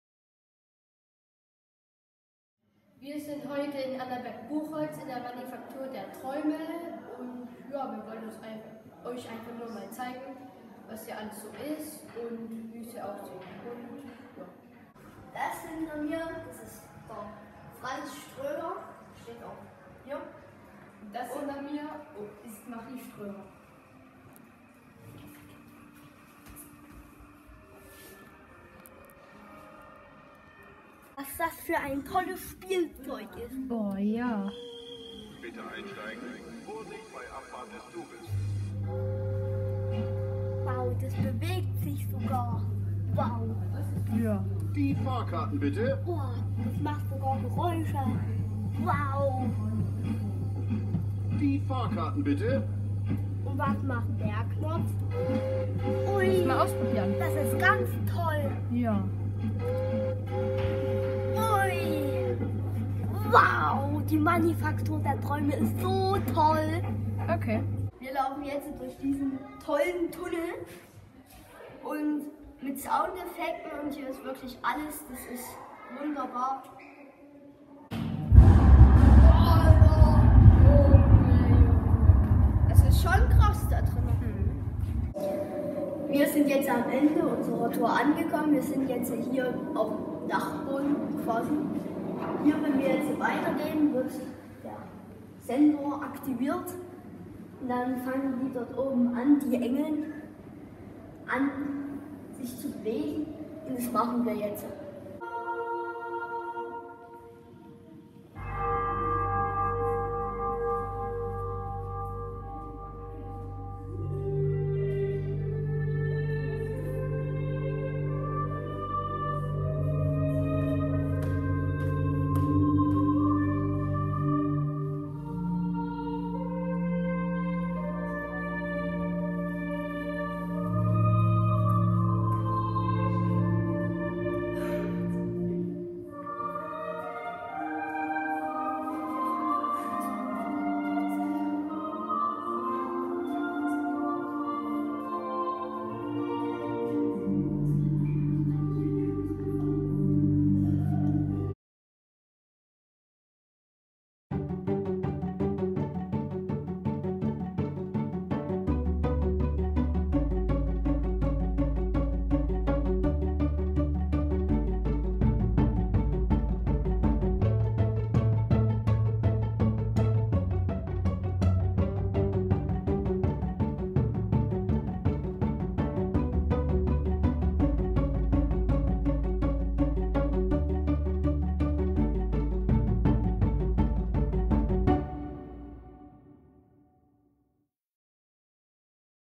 Manufaktur der Träume - MIC Erzgebirge-Mittelsachsen 2023 - 18
Schüler und Schülerinnen der Geschwister-Scholl-Oberschule Schönheide produzierten diesen Clip in der  MANUFAKTUR DER TRÄUME - Das Erlebnismuseum Annaberg-Buchholz.